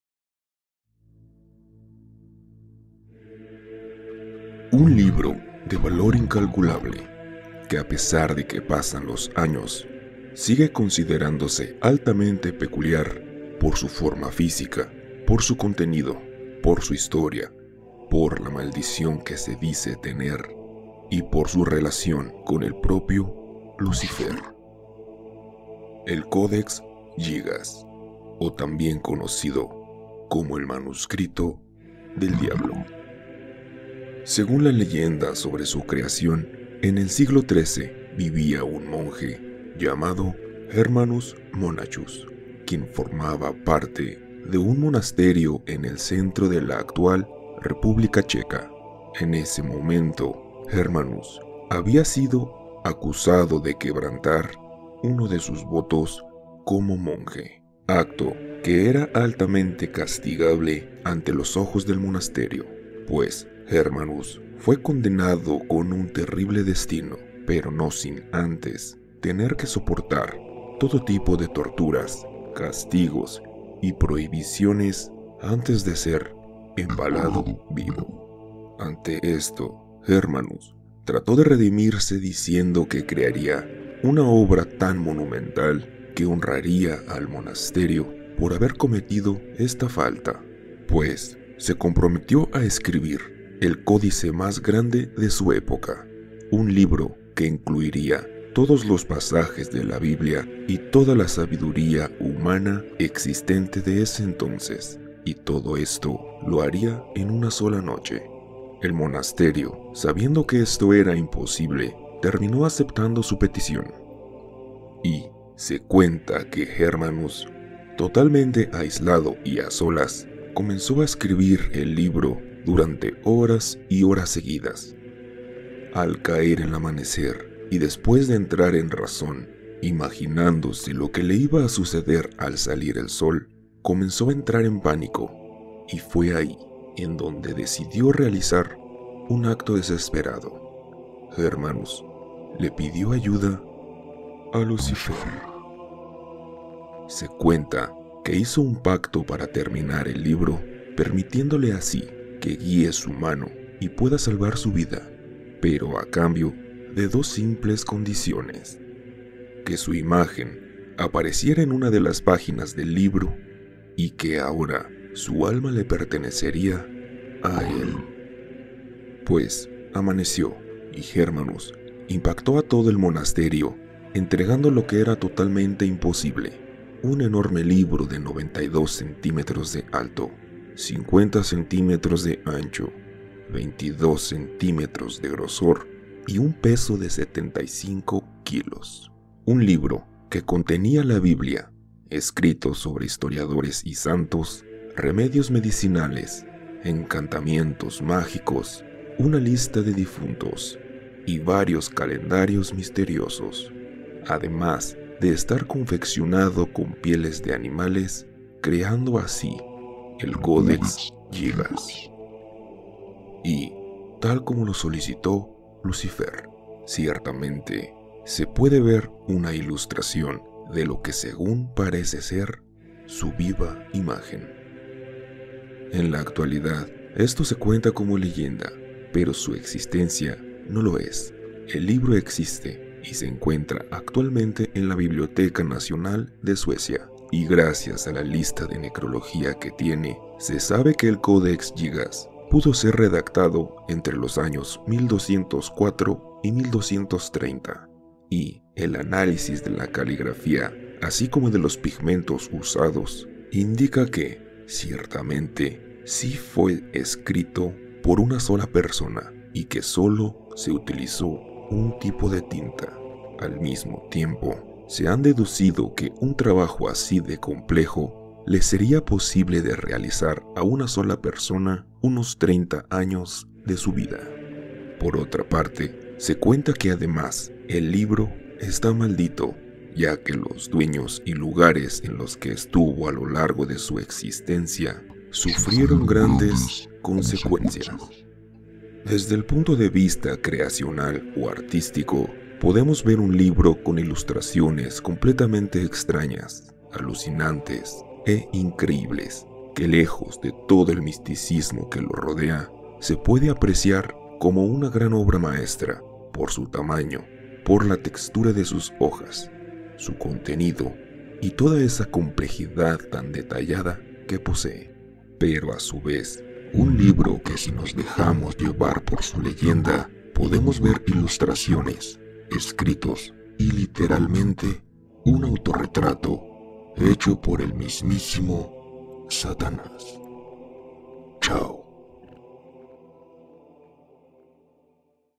El Codex Gigas | Un Extraño Libro Con Orígenes Oscuros..
Un libro muy peculiar considerado único, por su historia y contenido, además de guardar secretos, oscuros secretos..
Espero les agrade el video, gracias.